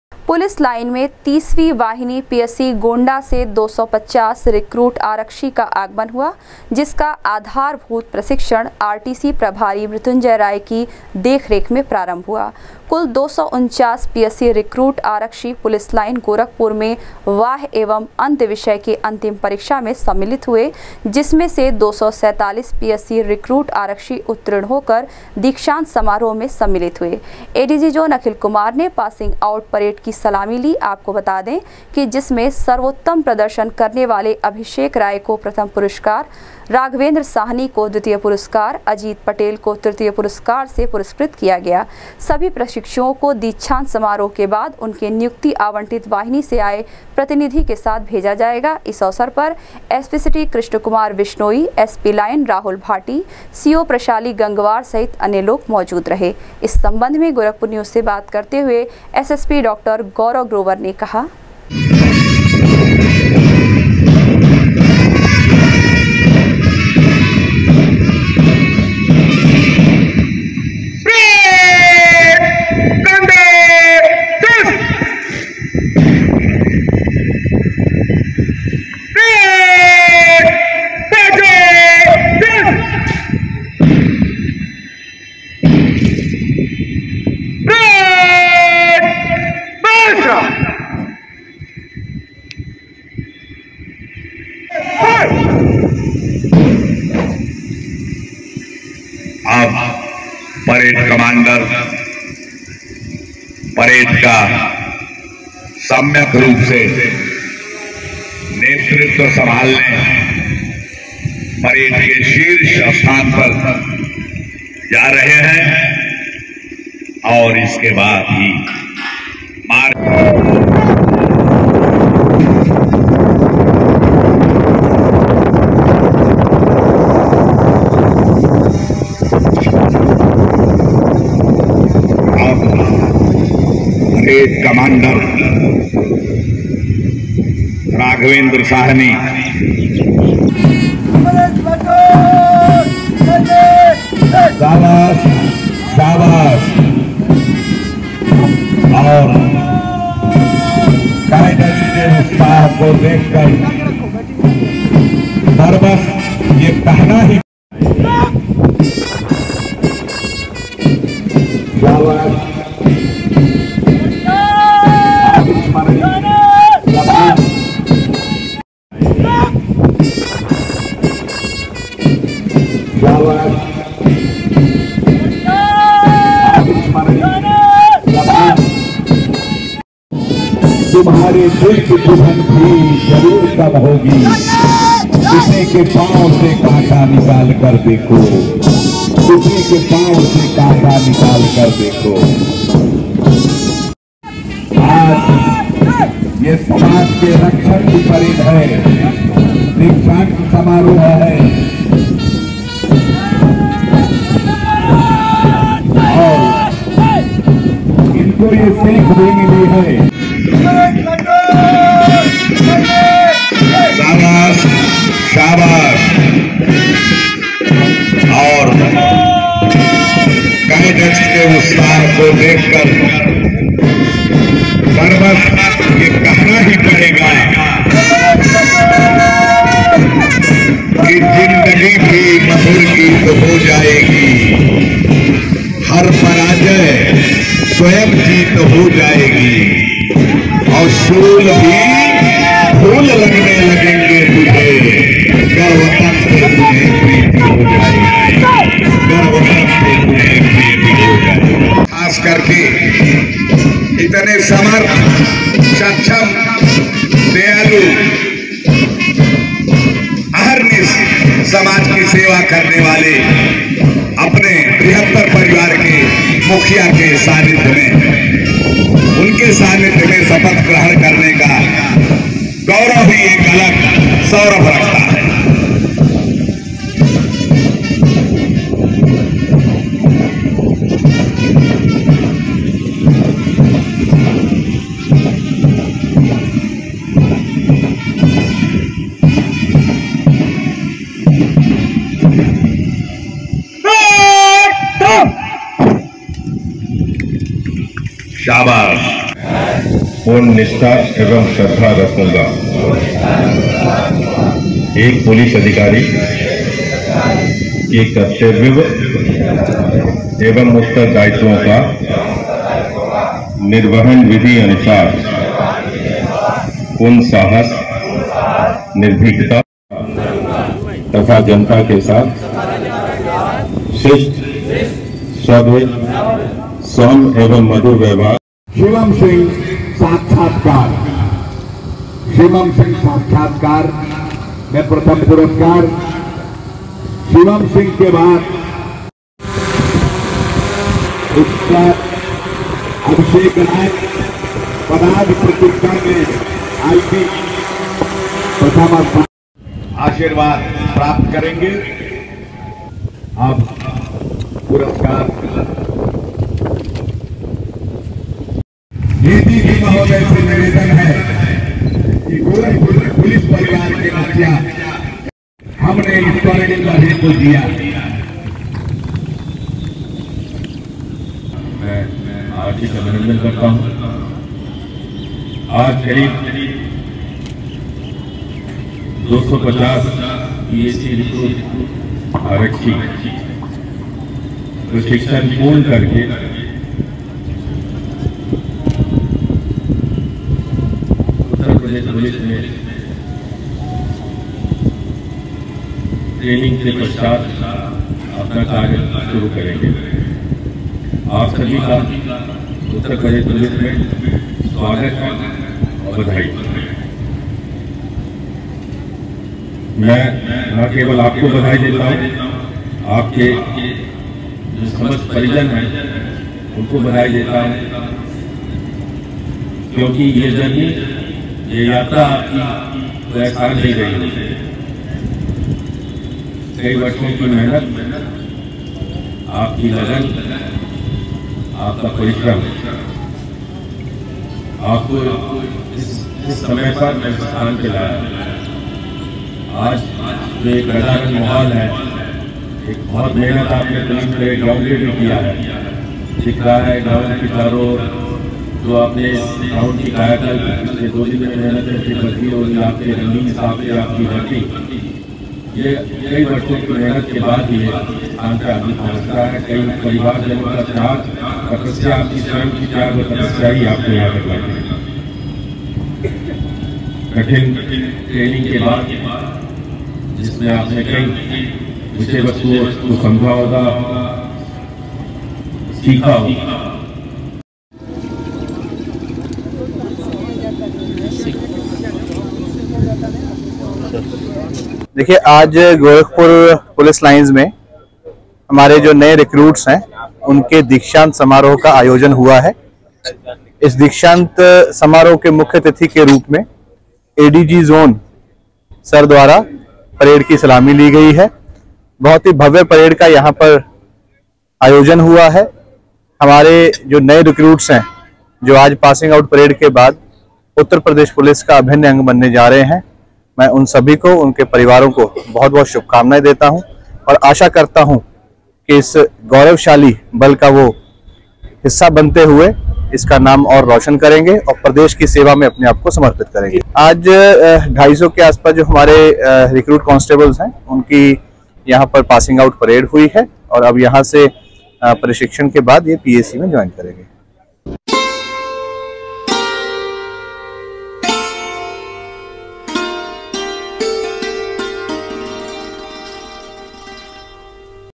गोंडा पीएसी से आये 250 रिक्रूट की ट्रेनिग खत्म होने के बाद पुलिस लाइन के परेड ग्राउंड पर दीक्षांत
गोंडा पीएसी से आये 250 रिक्रूट की ट्रेनिग खत्म होने के बाद पुलिस लाइन के परेड ग्राउंड पर दीक्षांत परेड का किया गया आयोजन,एडीजी जोन अखिल कुमार ने ली पासिंग परेड की सलामी,आरक्षियों को संयुक्त रूप से पद एवं कर्तव्यों का कराया शपथ ग्रहण,सभी के उज्ज्वल भविष्य की,की कामना